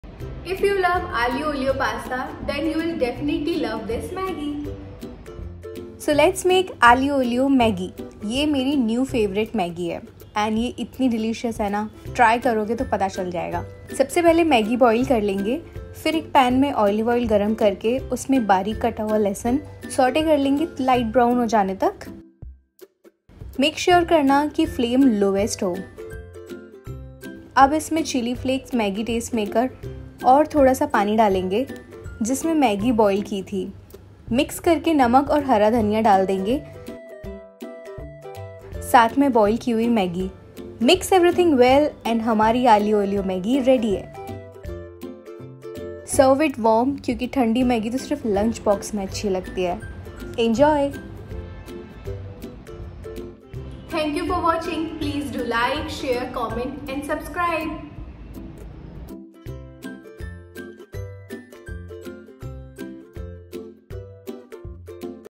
Aglio e olio Maggi | Fusion Maggi recipe | Garlicy & Spicy Maggi | Food By Priyanka
Aglio e olio maggi 🤤

Ingredients:
Chopped coriander leaves- handful
Chilli flakes-1 tsp
Maggi taste maker
Water we boiled maggi in-2 tbsp
Boiled maggi-1 cake